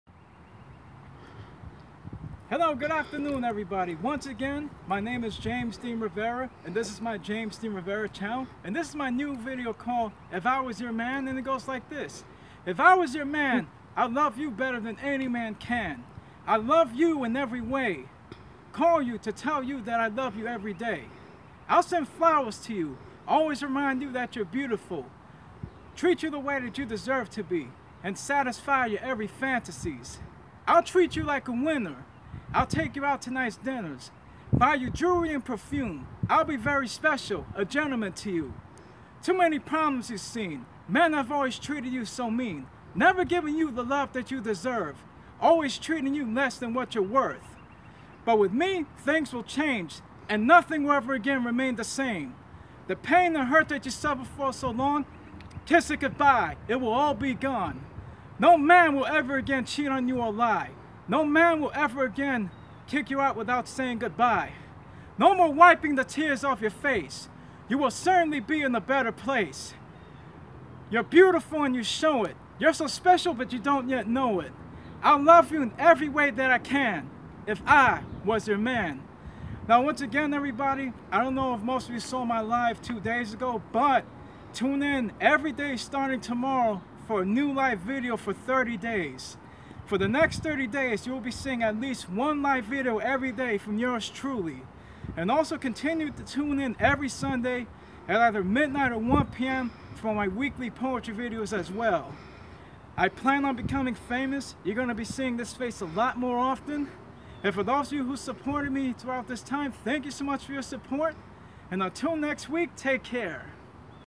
If I Were Your Man (Video 45)
A Poem about how better her life would be if I were her man